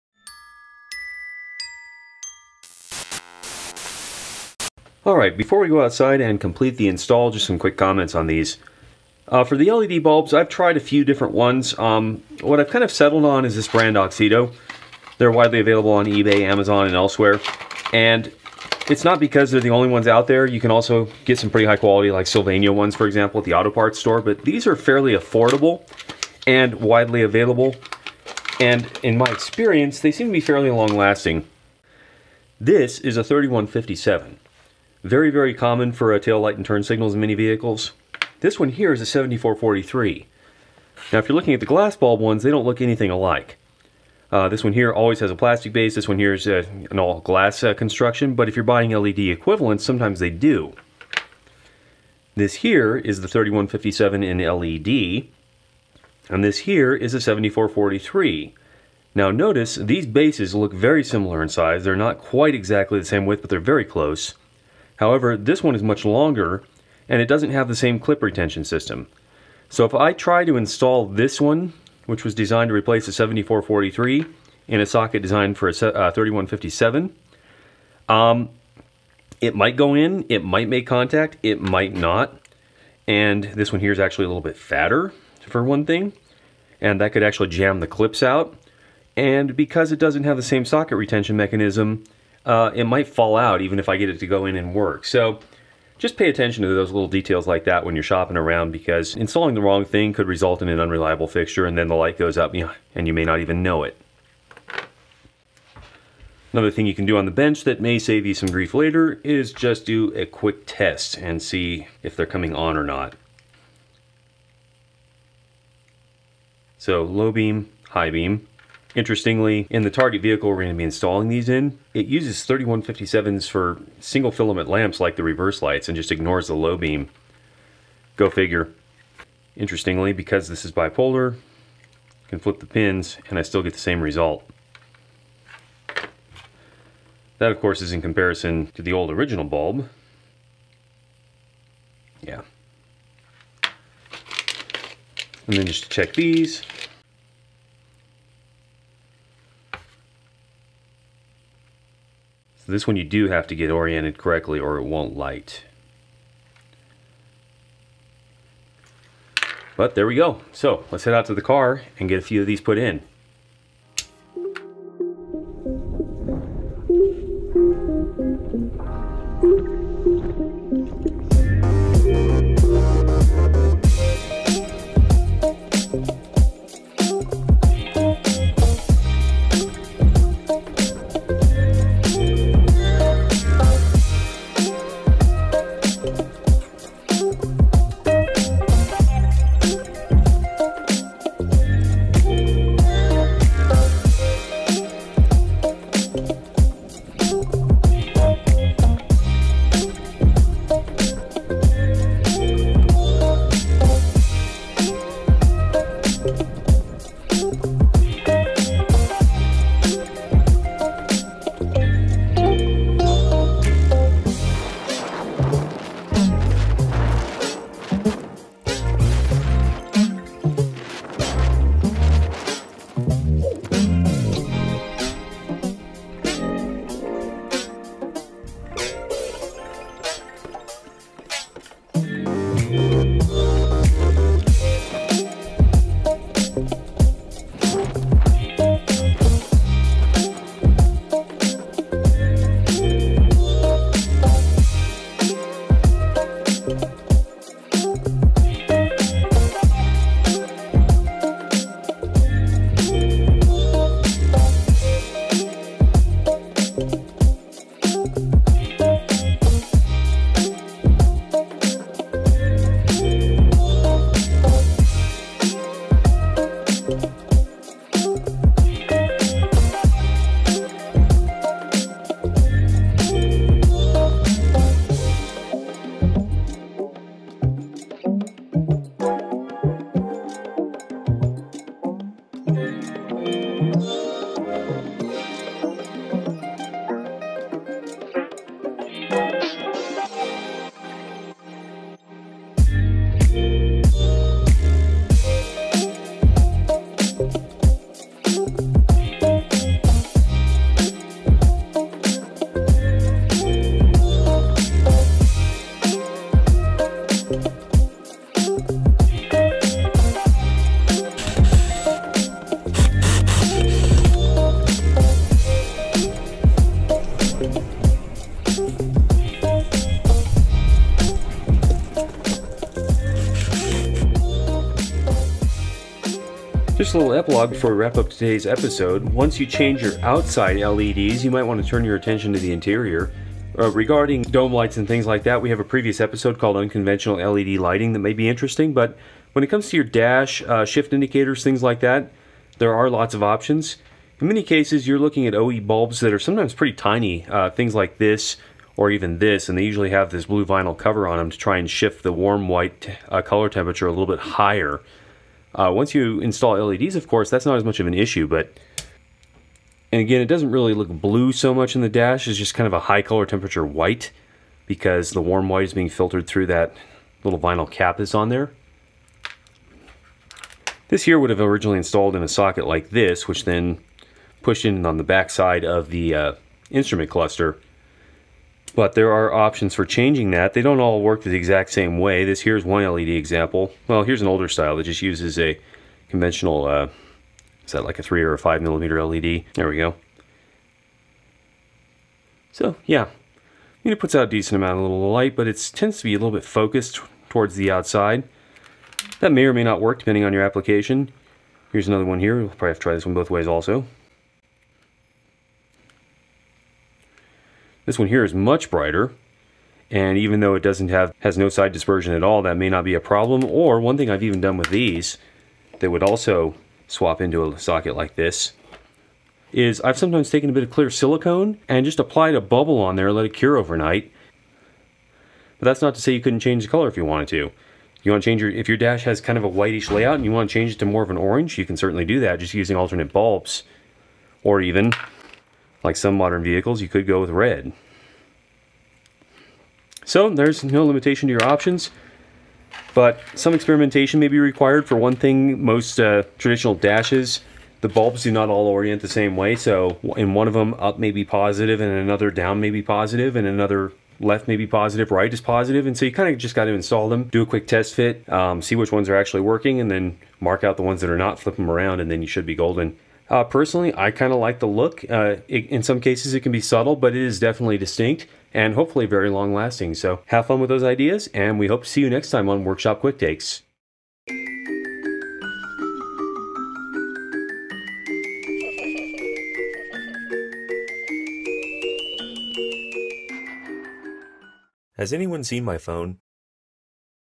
wqt//Basic Auto: LED exterior signal light conversions (1984-2001 Jeep Cherokee XJ)
Today on Workshop Quick Takes (wqt): If your vehicle uses incandescent lamps in the marker and signal lights, you may be ready for an LED upgrade. When properly selected, LED upgrades are brighter that the OE lamps, alert other drivers to your signals more quickly, and improve your vehicle's appearance.

We're going to look through a couple options today and then convert a 2000 Jeep Cherokee (XJ). Join us to examine some of your options!